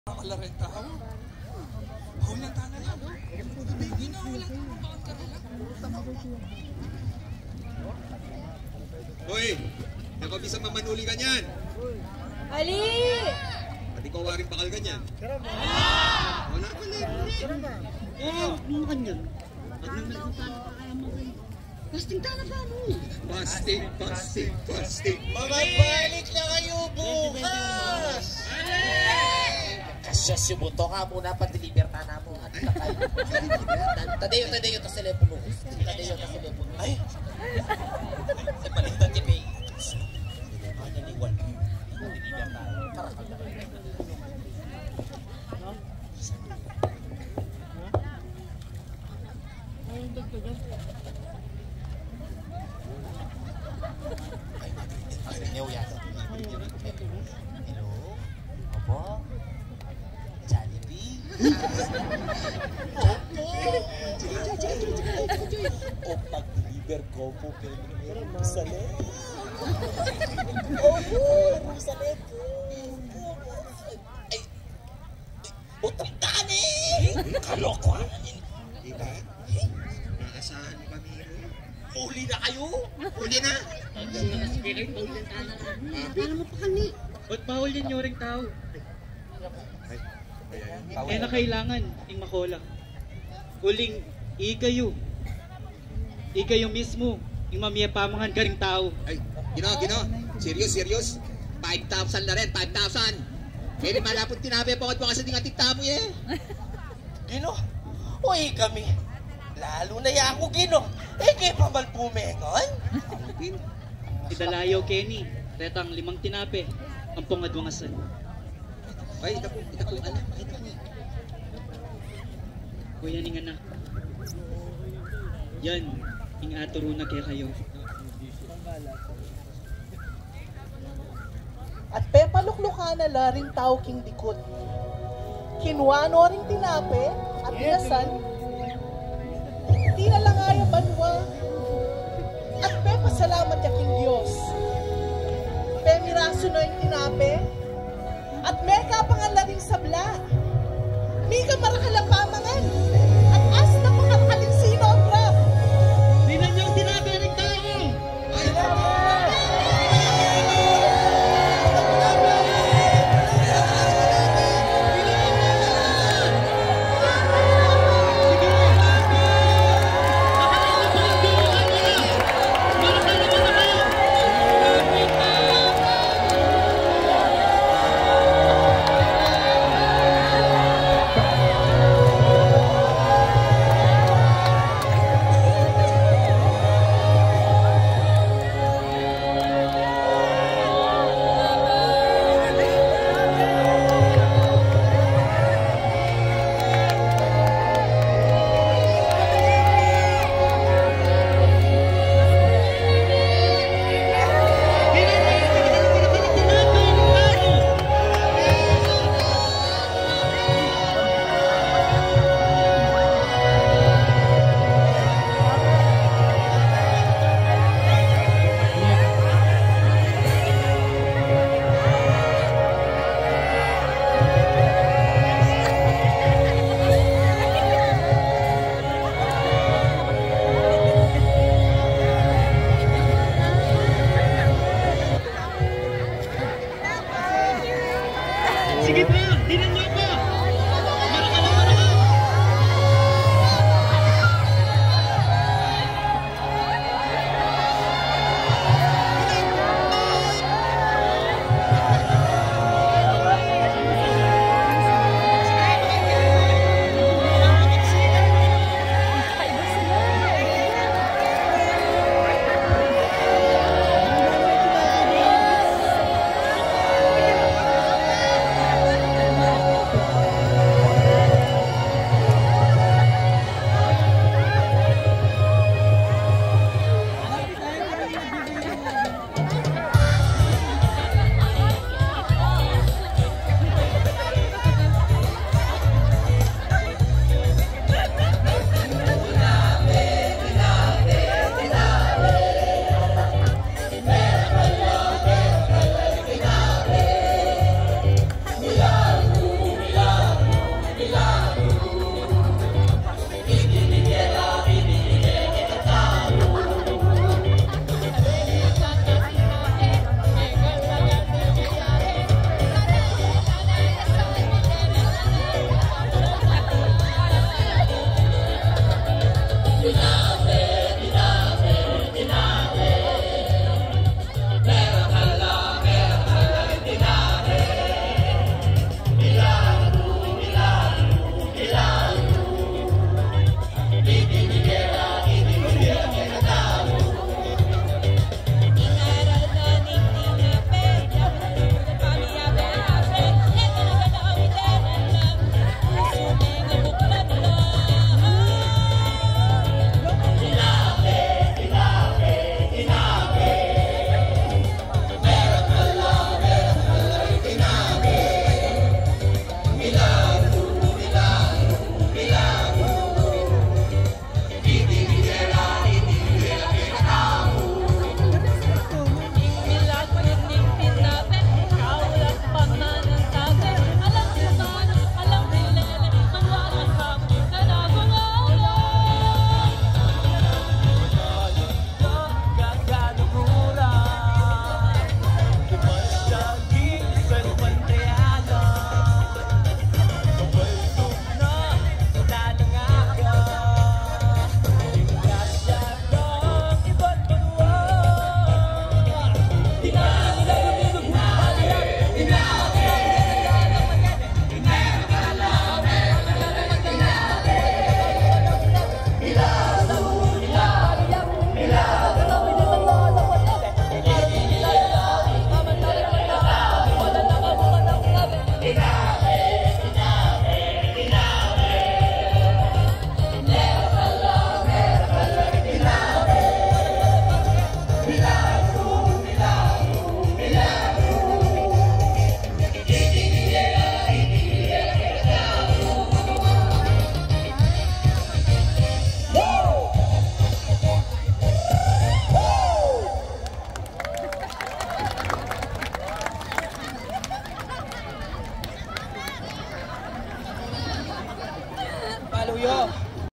Event: Tulauk 2024 08
Fancam video of Tulauk 2024, A Lenten Sarswela at Clark Parade Grounds, Pampanga, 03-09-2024